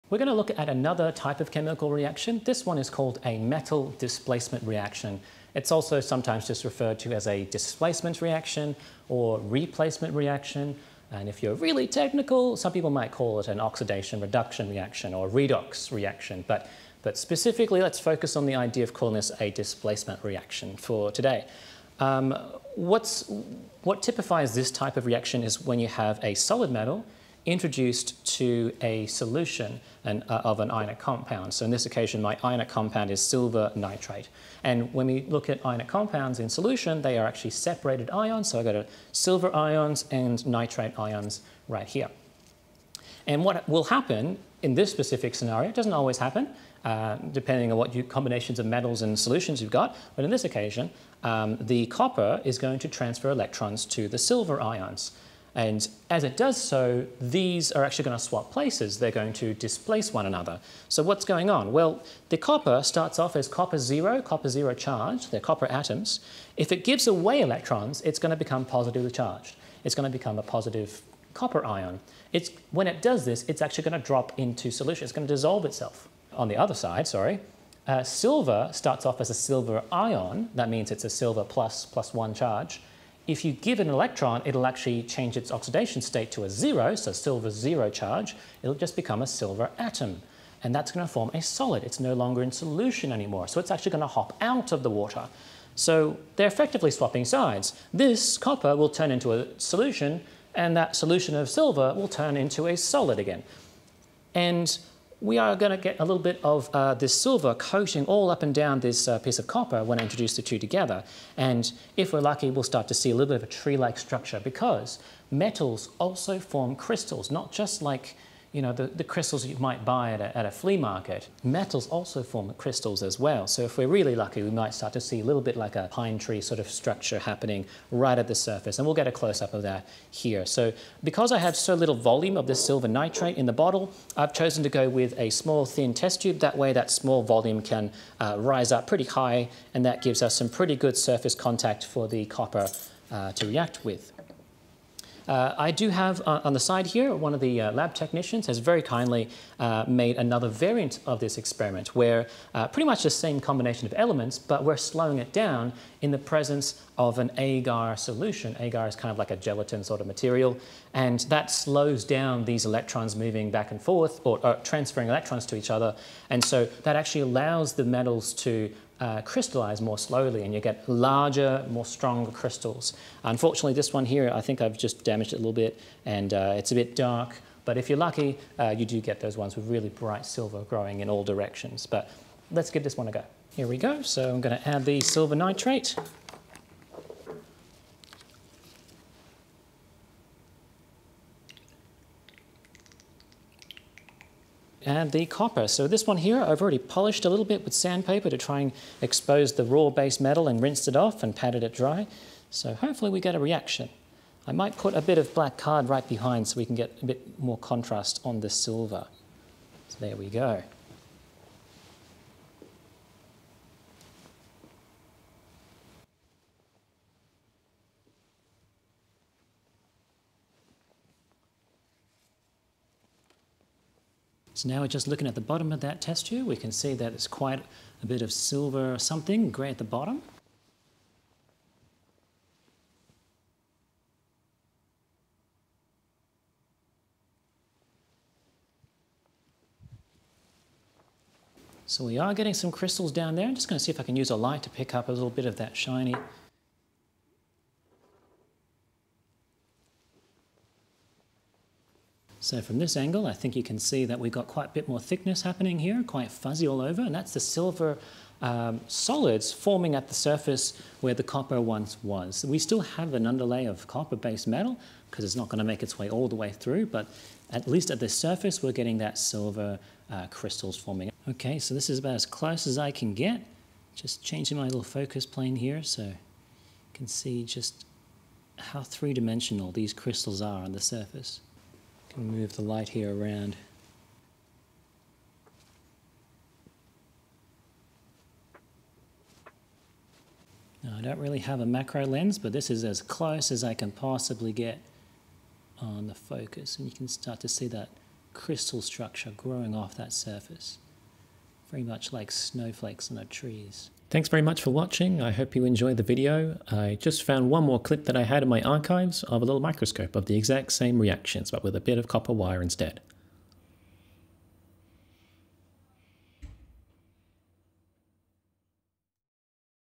Displacement Reactions
Discussing what displacement reactions are and a demonstration of growing silver crystals as an example.